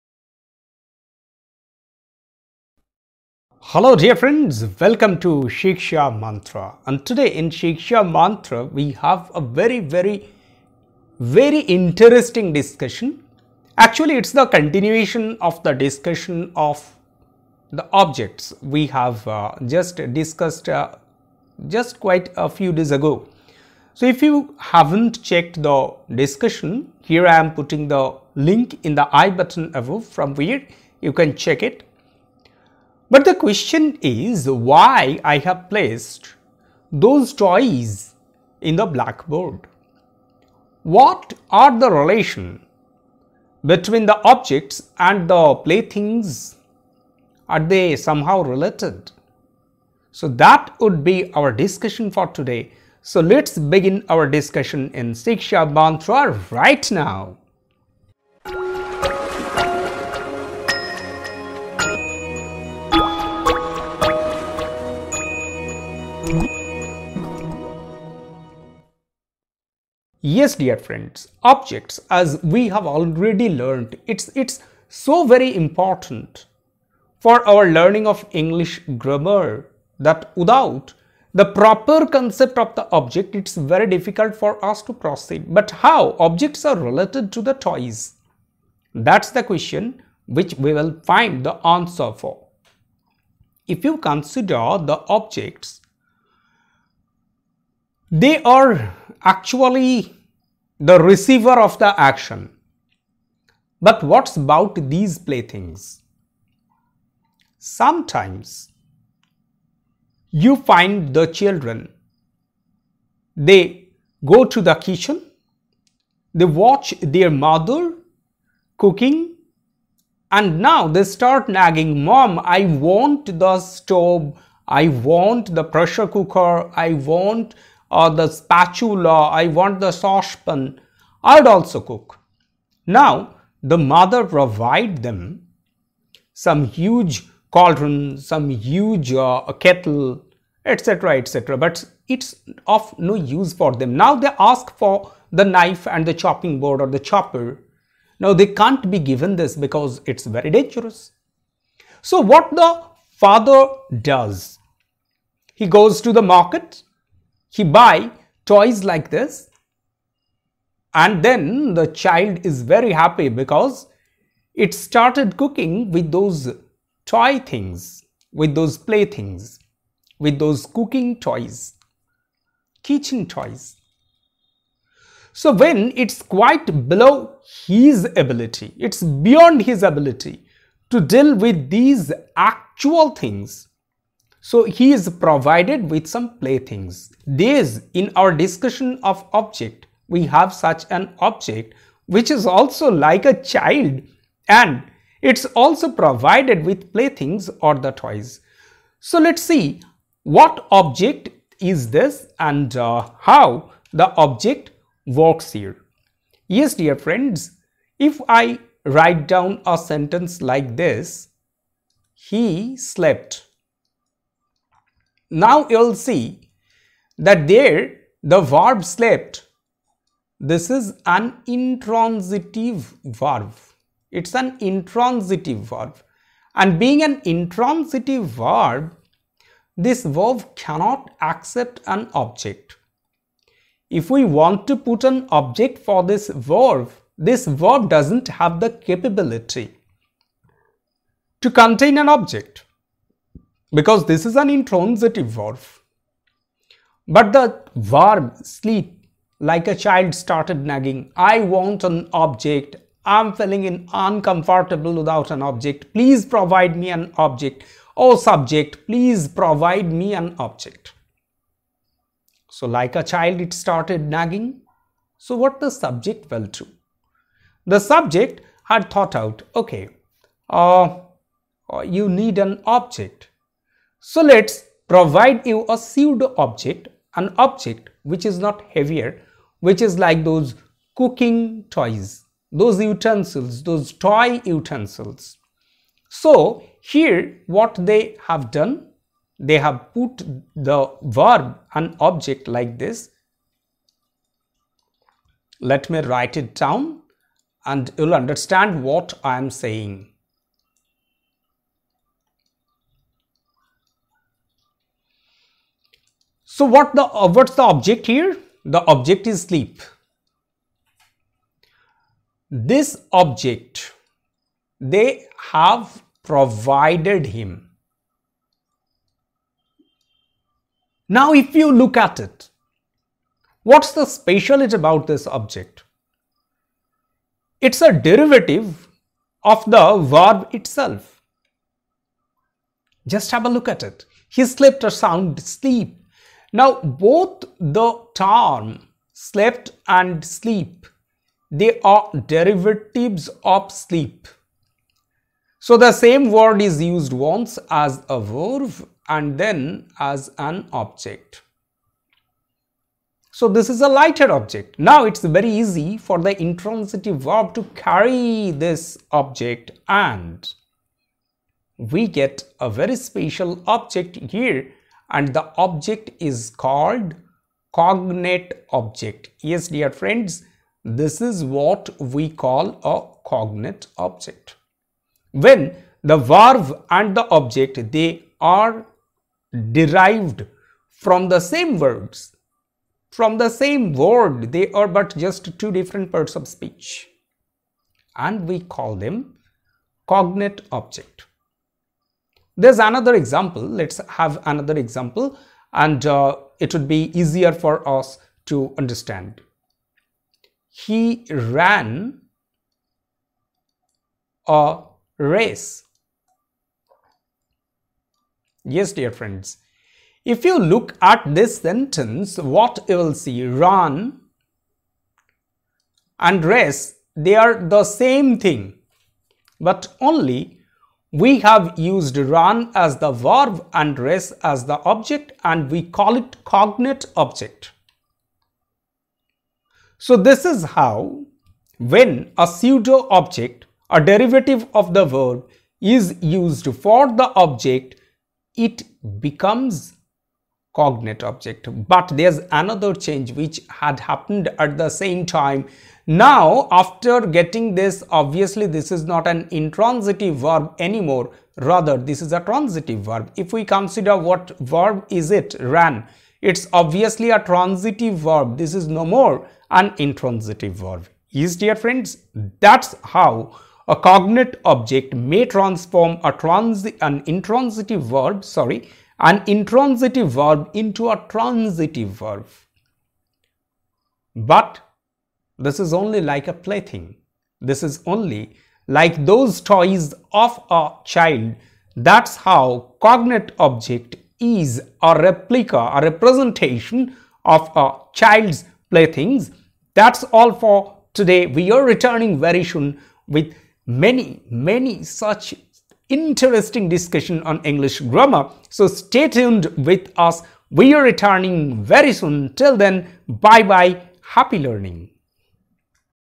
Cognate Objects Vs Child's Playthings
How the Cognate Objects are related to the playthings of a child?
Let's learn some small things regarding the language we are so fond of.
English grammar is fun if you really learn the tricks and tips of the language.